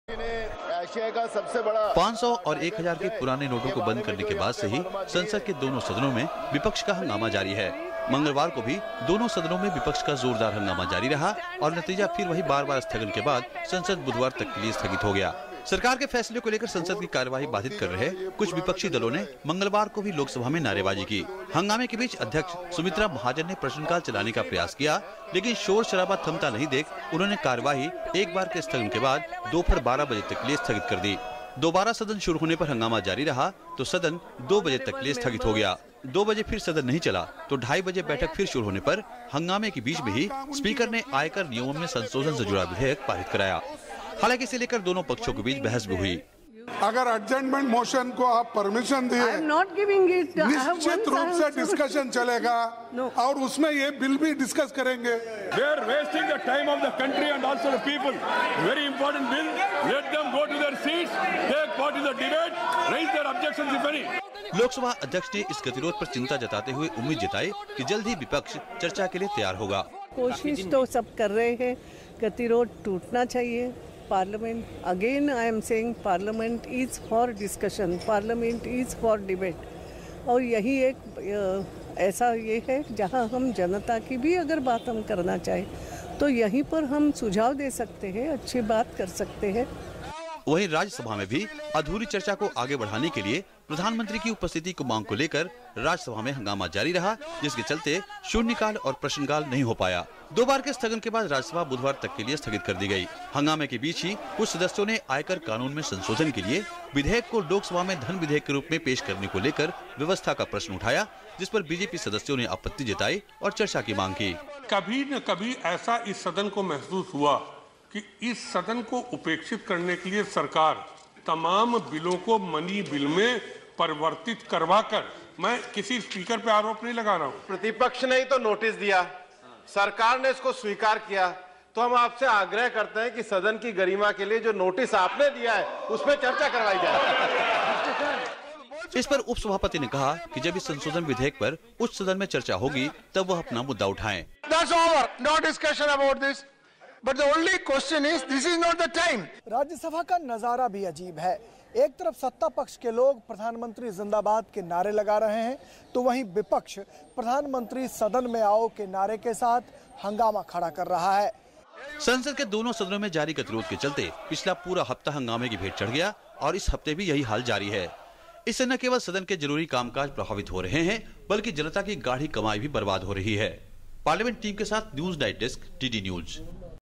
संसद में फिर हंगामा; लोकसभा में विपक्ष ने की नारेबाजी
बीते सप्ताह की तरह इस सप्ताह भी अभी तक संसद की कार्यवाही नही चल पा रही है विमुद्रीकरण के मुदद्दे पर हंगामा कर रहे विपक्ष के कारण न सिर्फ विधायी काम रुके पड़े है बल्कि जनता के पैसे की भी बर्बादी हो रही है।  हालांकि संसद में विपक्षी एकता टूट चुकी है तो सड़क पर सरकार को घेरने के प्रयास फ्लॉप हो चुके है।